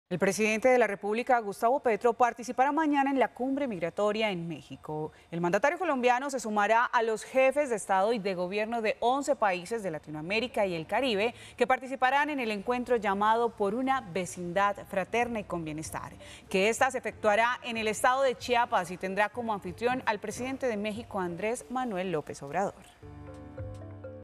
El presidente Petro participará en la cumbre migratoria en México
Será parte de los líderes de 11 países de Latinoamérica y el Caribe en el encuentro “Por una vecindad fraterna y con bienestar”, que se llevará a cabo en Chiapas.